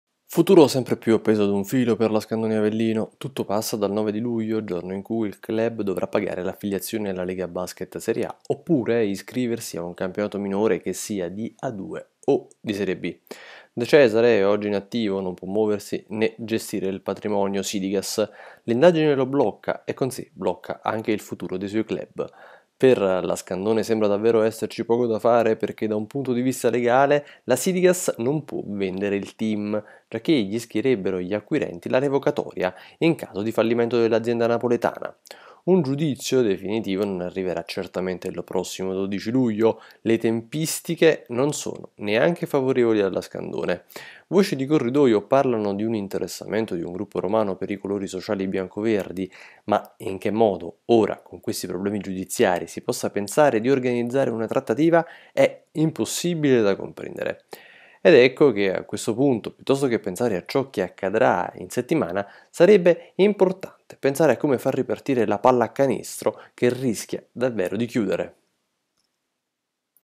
Scandone, cercasi acquirenti
Continua il caos in casa Scandone Avellino. Il futuro dei biancoverdi è appeso ad un filo, all'orizzonte un gruppo romano con cui il sindaco festa potrebbe incontrarsi, ma per ora resta tutto molto aleatorio